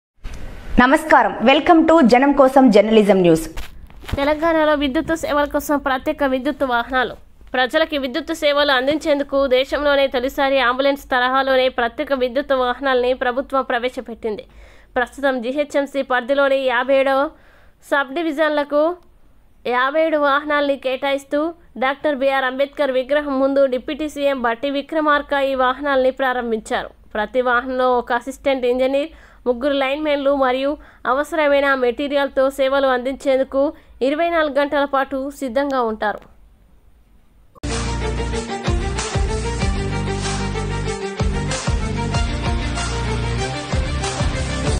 ప్రస్తుతం GHMC పరిధిలోని 57 సబ్ డివిజన్ లకు 57 వాహనాలను కేటాయిస్తూ డా. బి.ఆర్ అంబేద్కర్ విగ్ర...
ప్రస్తుతం GHMC పరిధిలోని 57 సబ్ డివిజన్ లకు 57 వాహనాలను కేటాయిస్తూ డా. బి.ఆర్ అంబేద్కర్ విగ్రహం ముందు డిప్యూటీ సీఎం భట్టి విక్రమార్క ఈ వాహనాలను ప్రారంభించారు. ప్రతి వాహనంలో ఒక అసిస్టెంట్ ఇంజనీర్, ముగ్గురు లైన్ మెన్లు మరియు అవసరమైన మెటీరియల్ తో సేవలు అందించేందుకు 24 గంటల పాటు సిద్ధంగా ఉంటారు.